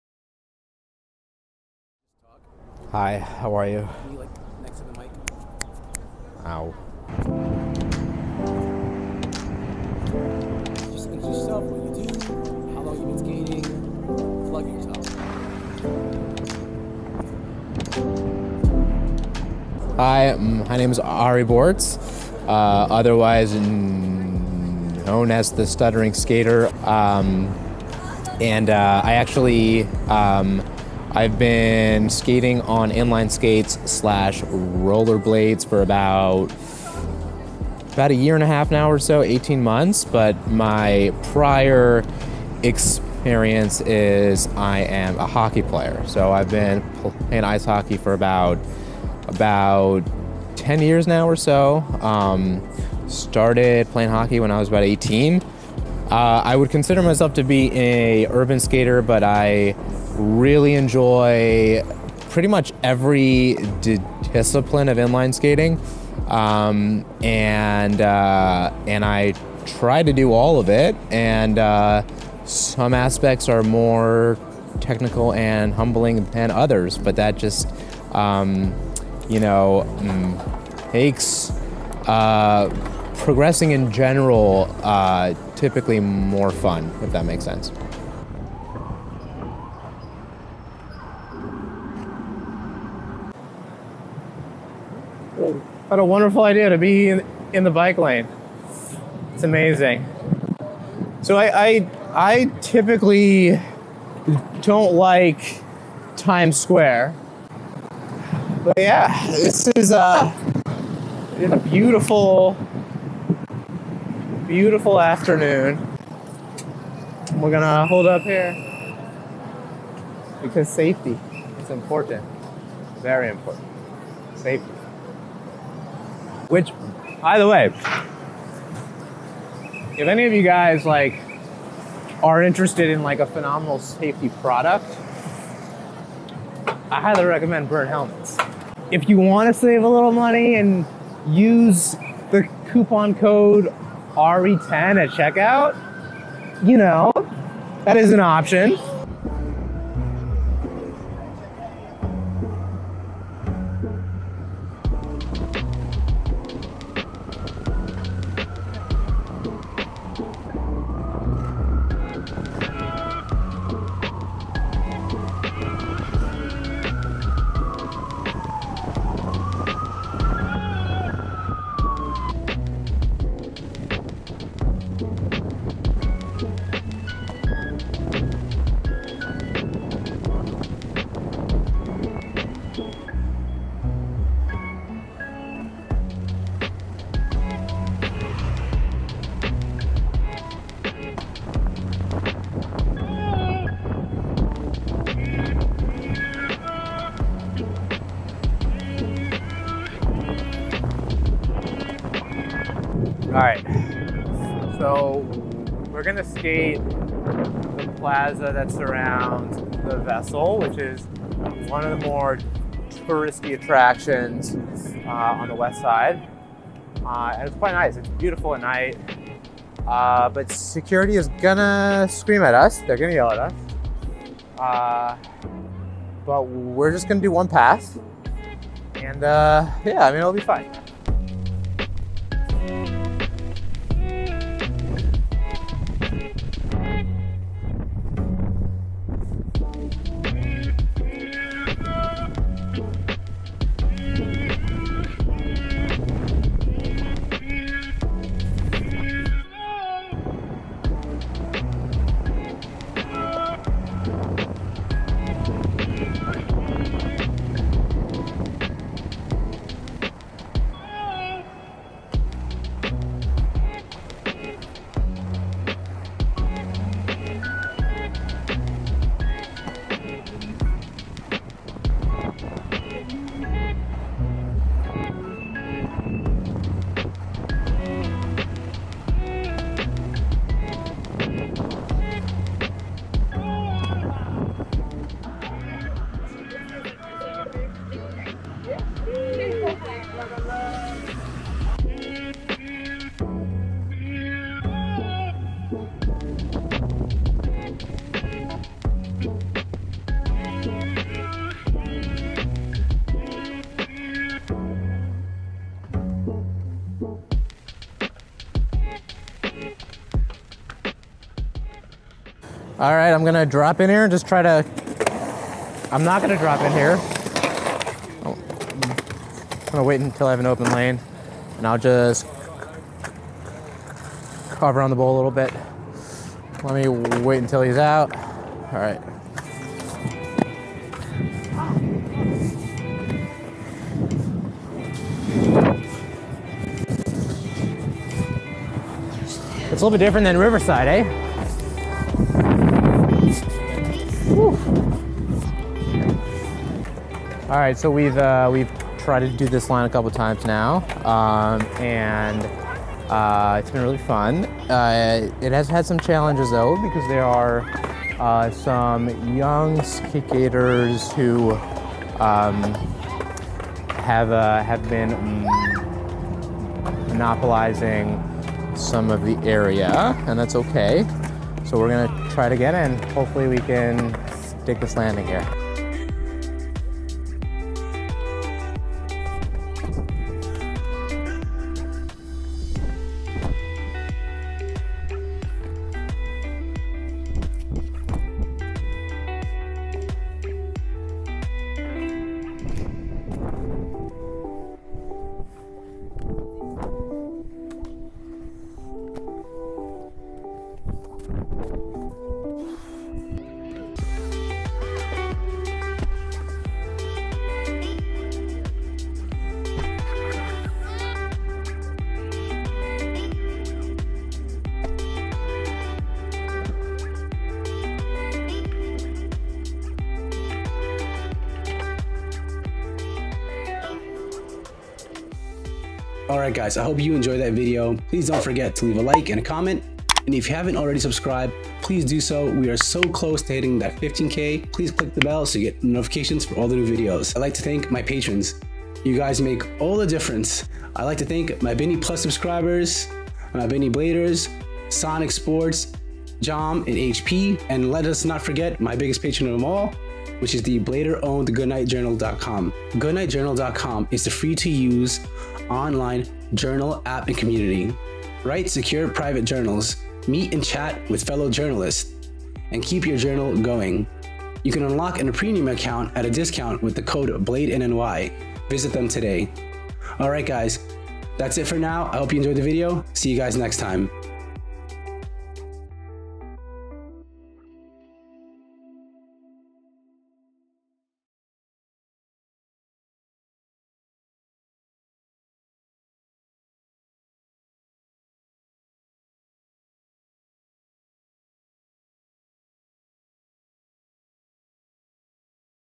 Blading With The Stuttering Skater
Join me as we skate with NYC's newest social media star The Stuttering Skater